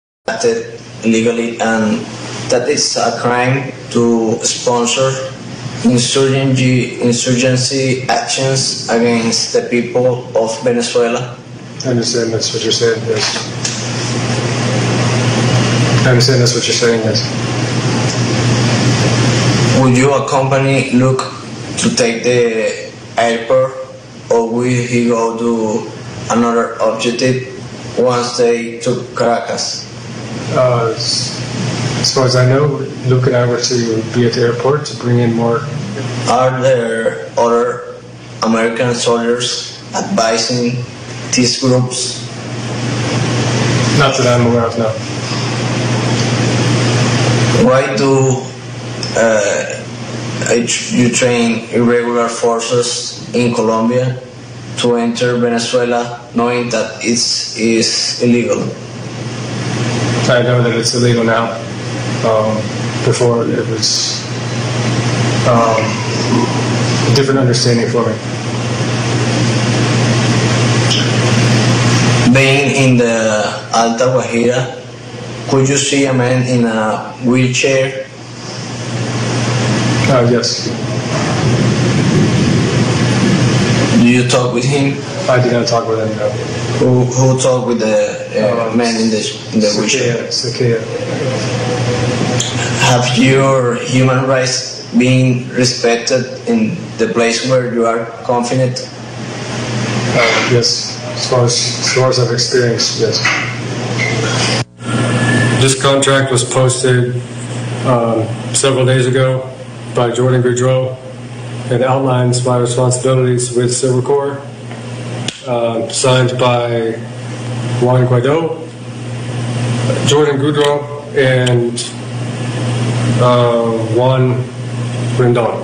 Confesión de Airan Berry, dando detalles de la Operación Gedeón 3/3
Confesión de Airan Berry, dando detalles de la Operación Gedeón, donde implica a Guaidó en la firma de un contrato con Jordan Grodeau